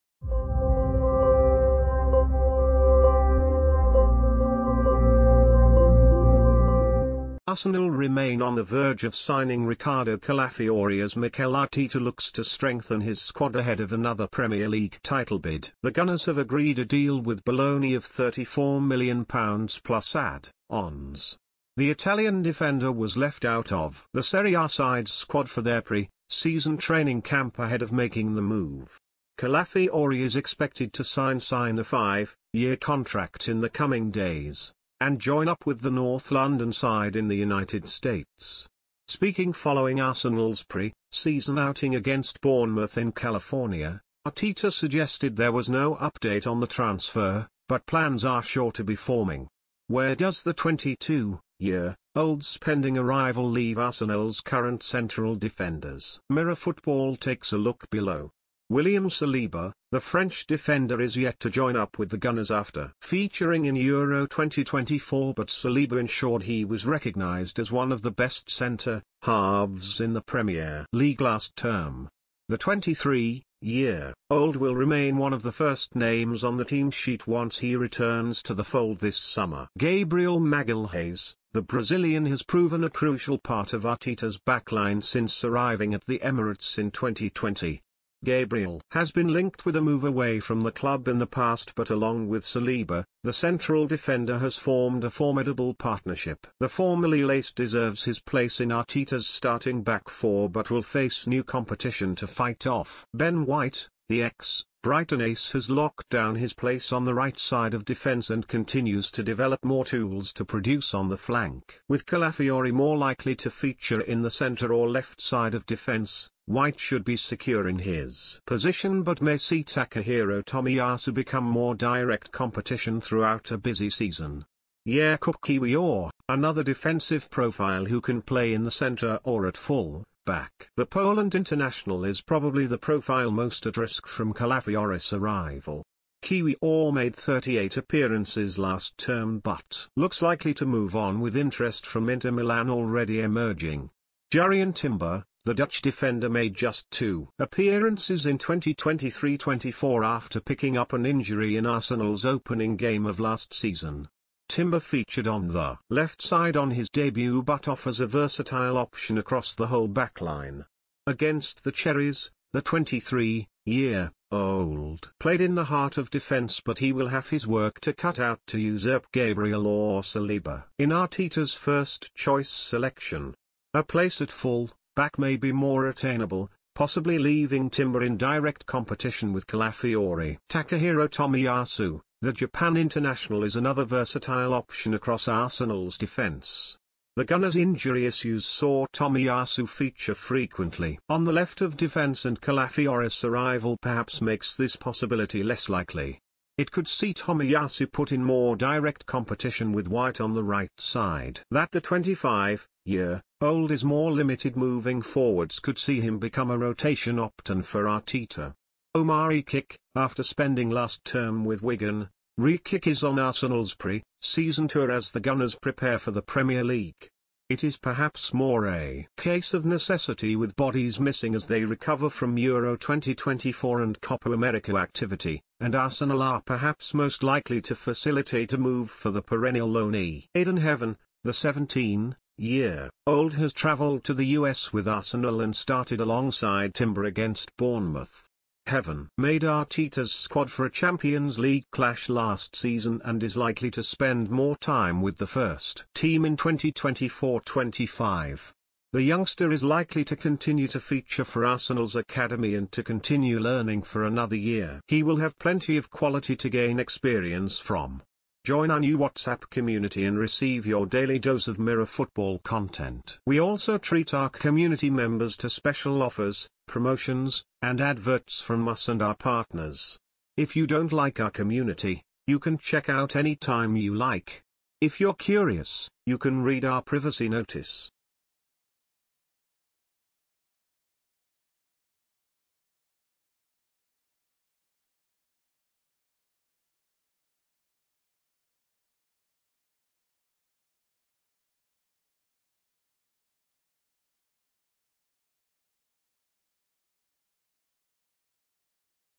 Arsenal FC News Now: Where Arsenal's eight centre-back options stand under Mikel Arteta as Ricc...
Arsenal FC News Now: Where Arsenal's eight centre-back options stand under Mikel Arteta as Riccardo Calafiori arrives. arsenal fc news now Arsenal remain on the verge of signing Riccardo Calafiori as Mikel Arteta looks to strengthen his squad ahead of another Premier League title bid. The Gunners have agreed a deal with Bologna of £34million plus add-ons. The Italian defender was left out of the Serie A side's squad for th. arsenal fc news now